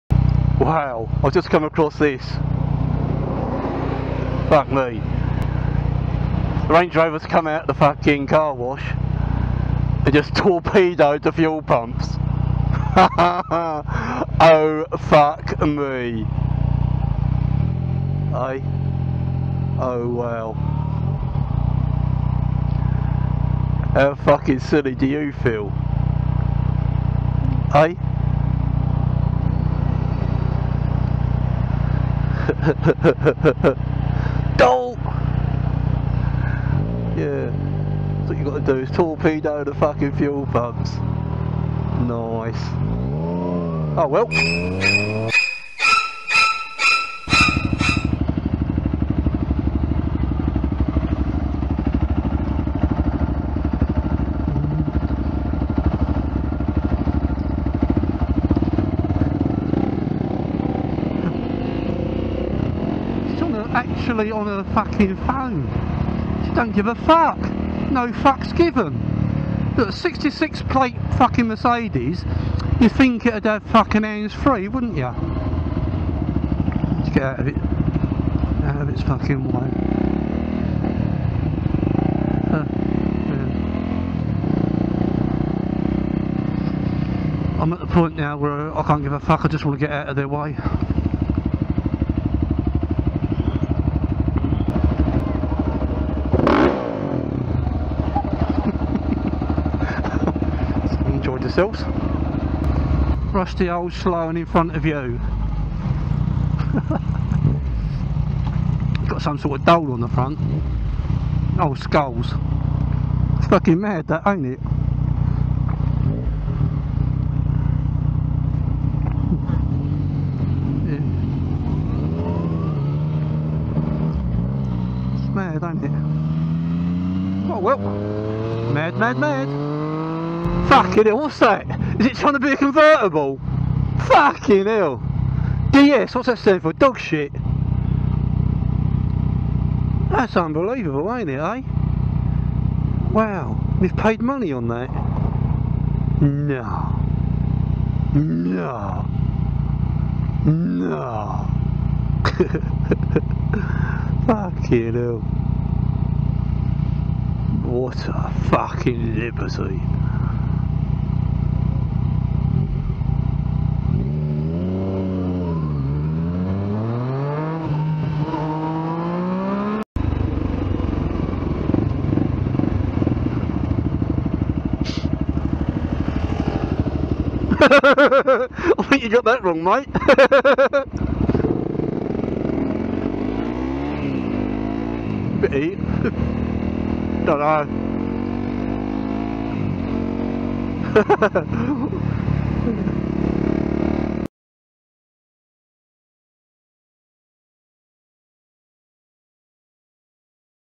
PP Rides London #24 London Observations
We go riding around London and these are the mad stuff we come across. I try to put a little sarcastic twist on all my vids, so please enjoy, comment and like..
Range Rover Crash
Girl on Phone
Rev Bomb Girls
VW Camper
Dog Shit Citroen
RLJ Scooter

MT-09 or FZ-09
Race Blue (Fastest Colour)
Full Race Carbon Akrapovic
Lots of little go faster bits.

Honda CRF 250L 2016
Red & White (Fastest Colour)
Leo Vince Exhaust
Lots of little go slower bits.

Shoei Helmet
Alpinestars Leathers, Boots and Gloves
Alpinestars One Piece race suit
Dainese Air Tex Jacket

Camera Garmin Virb